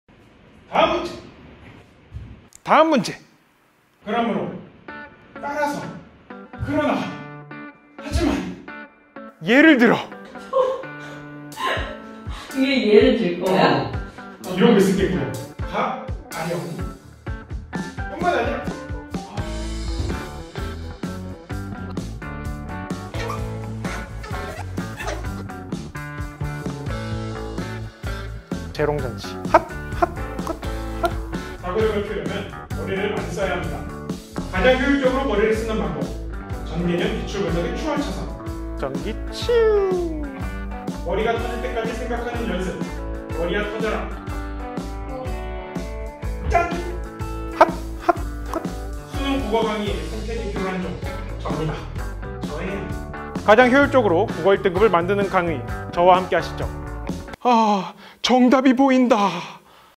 [오르비 클래스] 수능 국어계의 생태계 교란종
마음에 와닿은 이해황 선생님 강의 후기 툭.

일반적인 국어 강의가 아니라고 생각이 드네요.
이건 혁명이죠 .
사고의 힘이 무럭무럭 자랍니다.